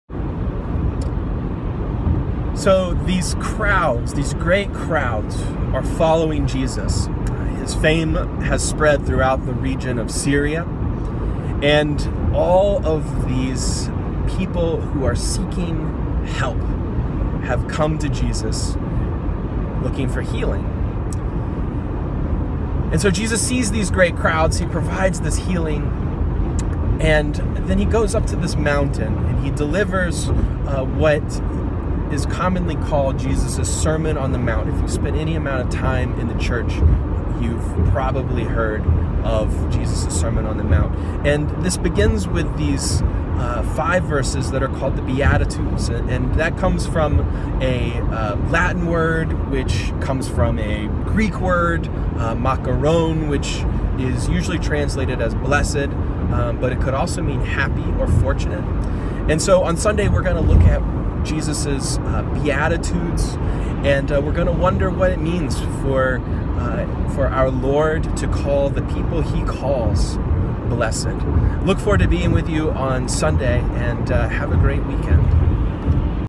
Sunday Preview 1/29/17
Sunday sneak peek from guest preacher Rev. Drew Stockstill!